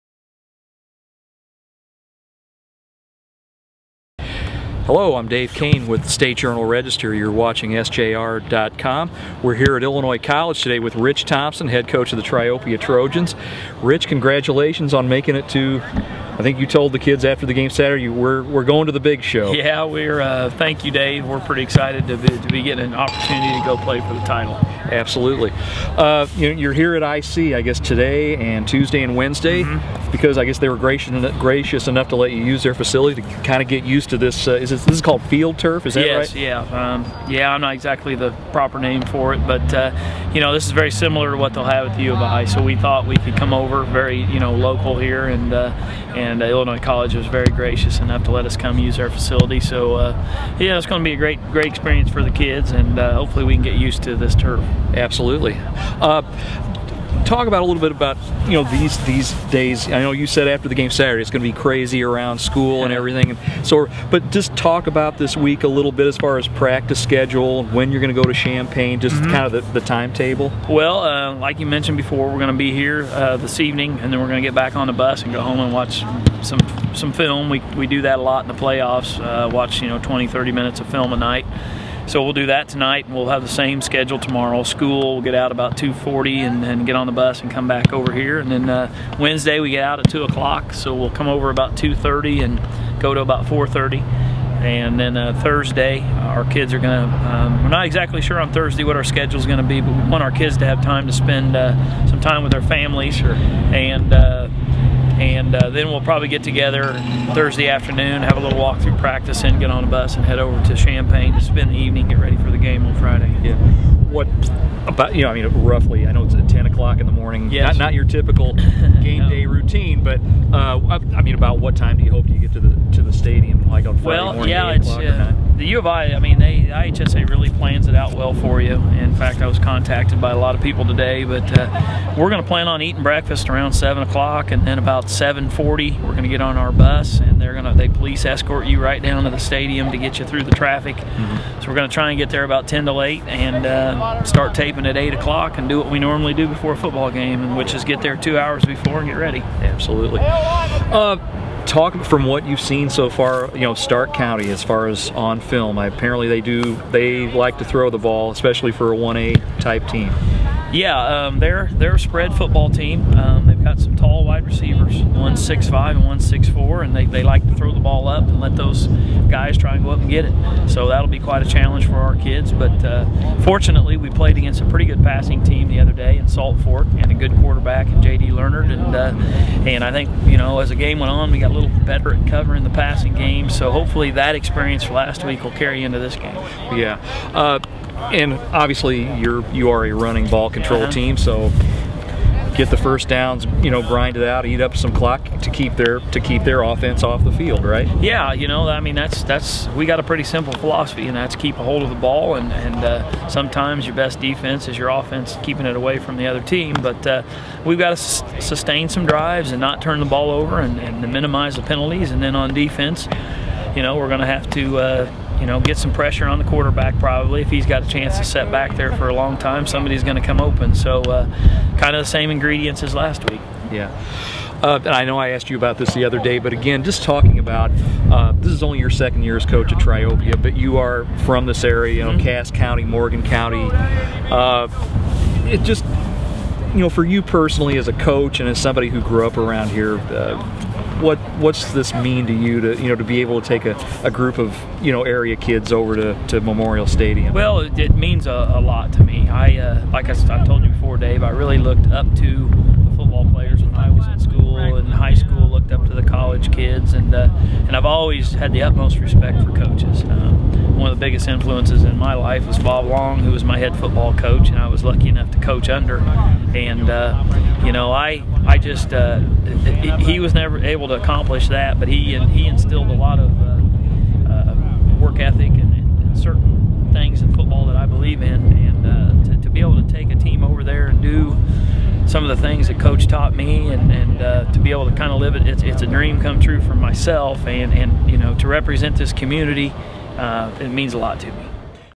Dave Kane with Triopia head coach Rich Thompson
Dave Kane talks with Triopia's head coach Rich Thompson Class 1A state championship at 10 a.m. Friday in Champaign.